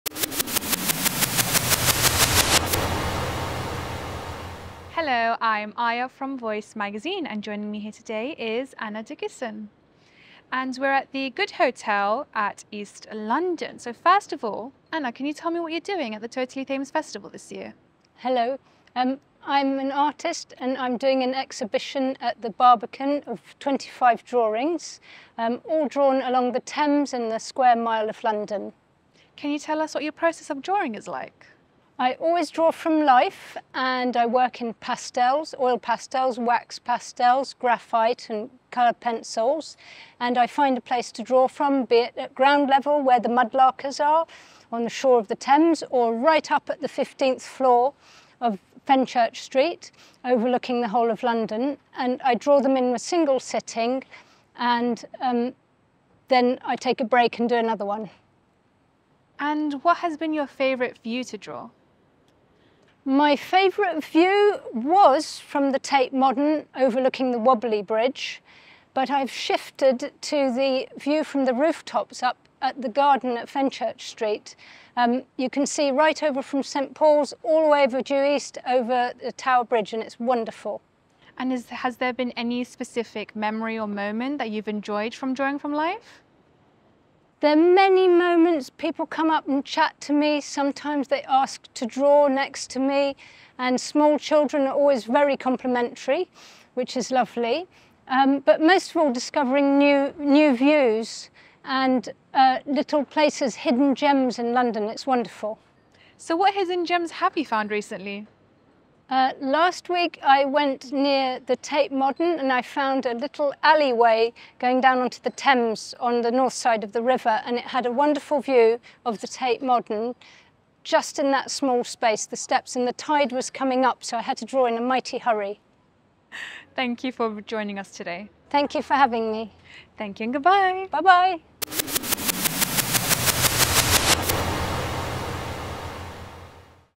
Nano-chats: London: Skyline and River @ Totally Thames 2023
Ayah from Voice spoke to Anna Dickerson about her solo exhibition of London cityscape paintings and drawings at the Barbican Library Gallery Space.

Team: Ayah Khan | Tim Day | Voice Magazine
Participant: Anna Dickerson
Venue: The Good Hotel
Supported by: Port of London Authority | Team London Bridge | The Royal Docks